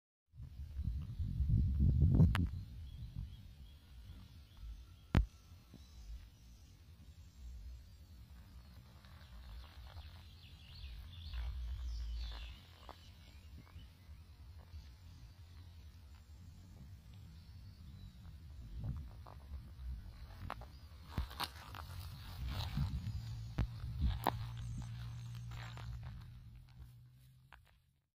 A Little cute Hedgehog in a natural environment.
A hedgehog is cute spiny mammal which can be domesticated as well.